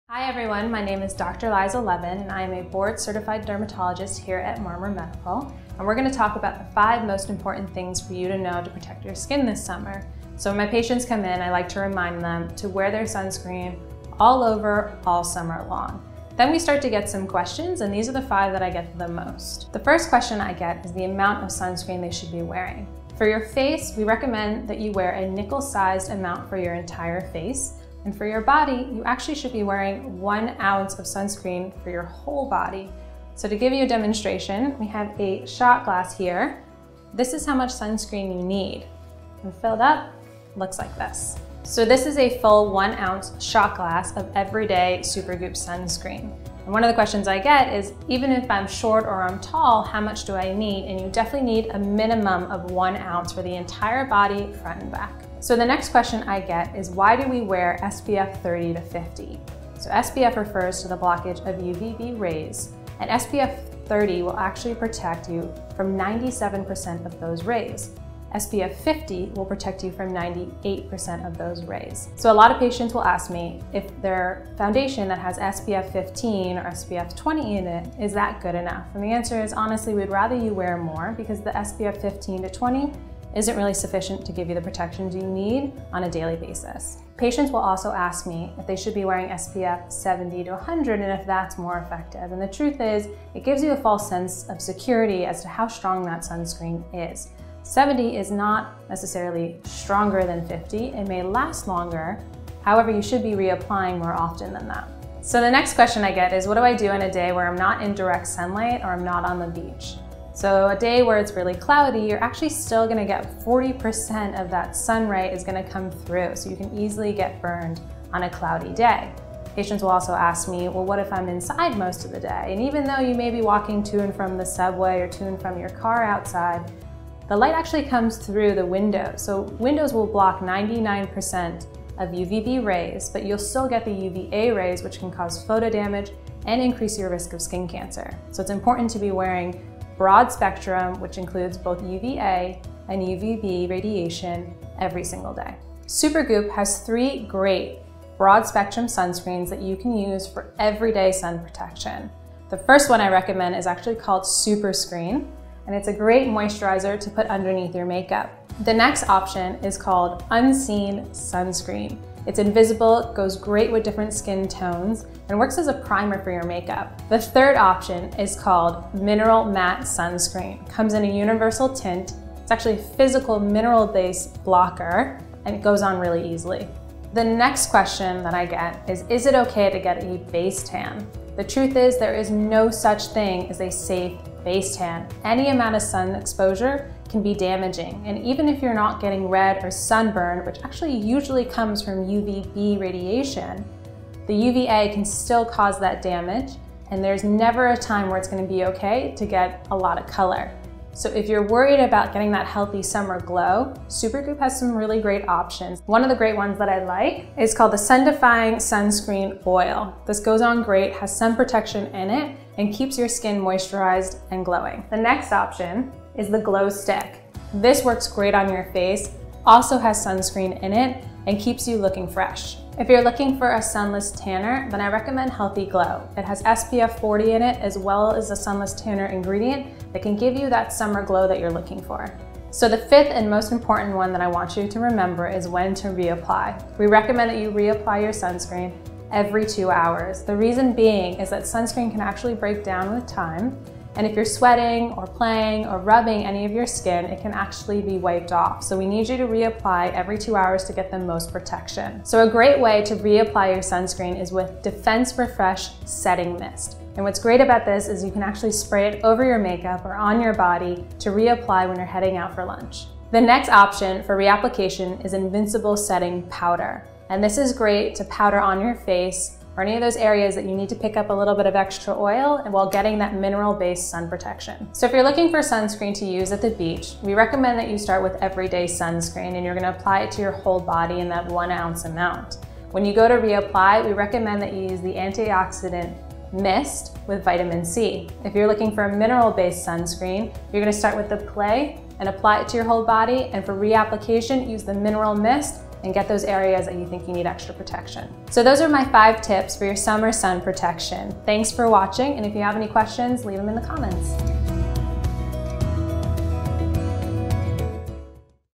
Learn From a Derm: 5 Things You Should Know About Protecting Your Skin This Summer
Welcome to Learn From a Derm, our new series where we have our favorite dermatologists talk about the things you need to know when it comes to your skin and SPF.
Our friend and board-certified,NYC-based dermatologist Dr. Liza Levin, is sharing the five biggest questions about SPF she gets in the summertime. Learn about how much you should be applying, the right SPF number to use and the best ways to reapply - then go have lots of fun in the sun.

Follow Supergoop!